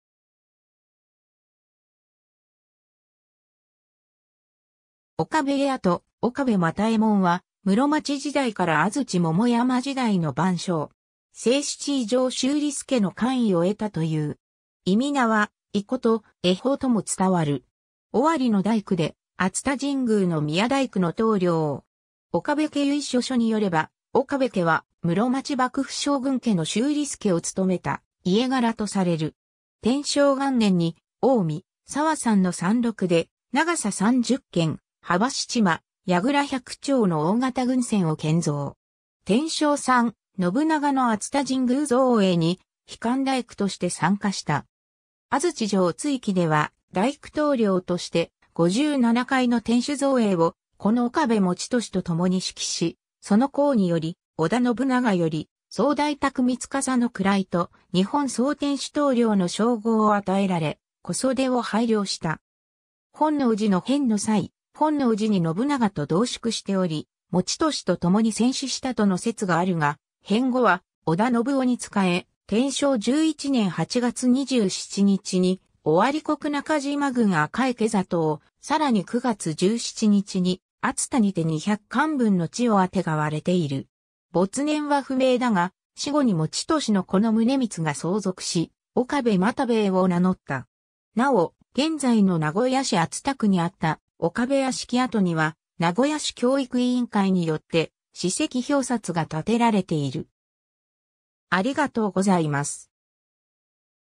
岡部又右衛門
岡部家跡（名古屋市熱田区） 岡部 又右衛門（おかべ またえもん、生没年不詳）は、室町時代から安土桃山時代の番匠。
正七位上修理亮の官位を得たという。
諱は以言、吉方とも伝わる。
 尾張の大工で、熱田神宮の宮大工の棟梁。
『岡部家由緒書』に拠れば、岡部家は室町幕府将軍家の修理亮を勤めた家柄とされる。
 天正元年（1573年）に近江・佐和山の山麓で長さ30間、幅7間、櫓100挺の大型軍船を建造（『信長公記』巻六）。
天正3（1575年）、信長の熱田神宮造営に被官大工として参加した。
 安土城築城では大工棟梁として、5重7階の天守造営を子の岡部以俊（岡部又兵衛）と共に指揮し、その功により織田信長より「総大匠司」の位と「日本総天主棟梁」の称号を与えられ小袖を拝領した。
 本能寺の変の際、本能寺に信長と同宿しており、以俊と共に戦死したとの説があるが、変後は織田信雄に仕え、天正11年（1583年）8月27日に尾張国中島郡赤池郷を、さらに9月17日に熱田にて200貫文の地を宛がわれている（張州雑志・分限帳）。
没年は不明だが、死後に以俊の子の宗光が相続し、「岡部又兵衛」を名乗った。
 なお、現在の名古屋市熱田区にあった岡部屋敷跡には名古屋市教育委員会によって史跡表札が立てられている。